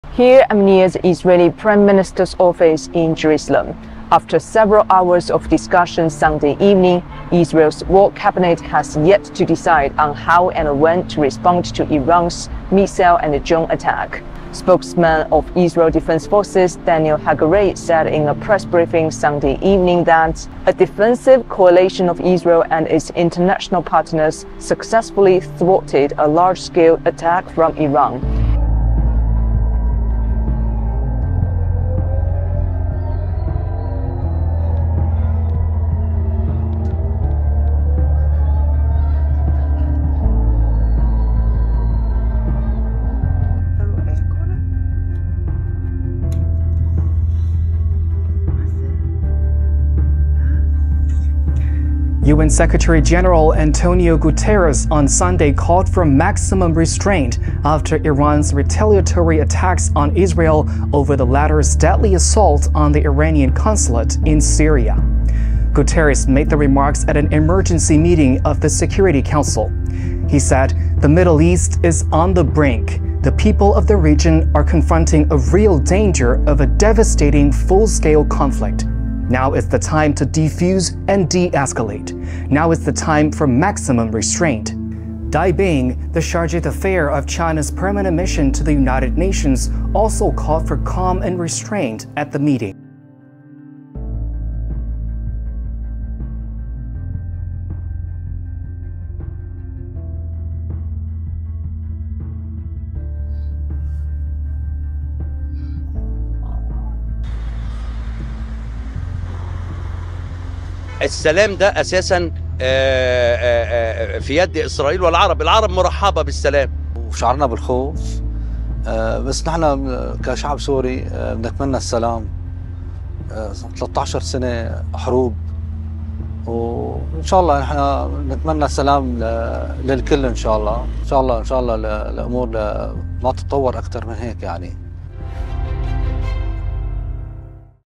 GLOBALink | Int'l community calls for de-escalation amid Iran-Israel tension
The international community has urged restraint among related parties to prevent a further escalation of the situation in the Middle East following Iran's retaliatory strikes on Israel.